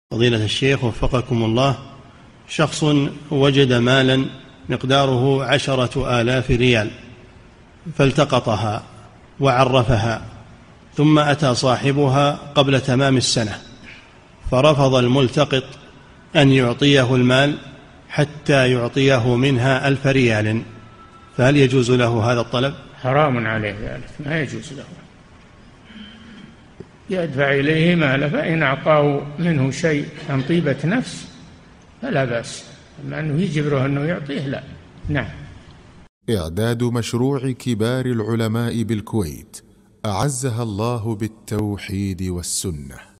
[2655 -3022] رجل وجد مالا فعرَّفها فجاء صاحبها فرفض المعرف أن يُعطيه المال حتى يقسم له نصيبا منها!
-"[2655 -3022] السؤال: شخصٌ وجد مالاً مقداره عشرة آلاف ريال فالتقطها وعرَّفها ثم أتى صاحبها قبل تمام السنة فرفض الملتقط أن يُعطيه المال حتى يُعطيه منها ألف ريالٍ فهل يجوز له هذا الطلب؟
الجواب: الجَوَابُ: حرامٌ عليه ذلك، ما يجوز له، يدفع إليه ماله، فإن أعطاه منه شيء عن طيبة نفس، فلا بأس، أَمَّا أنه يجبره أنه يعطيه، لا.
للاشتراك في قناة الشيخ صالح الفوزان

للاستفسار والإقتراحات والدعم
رئيس الفريق
لمشروع كبار العلماء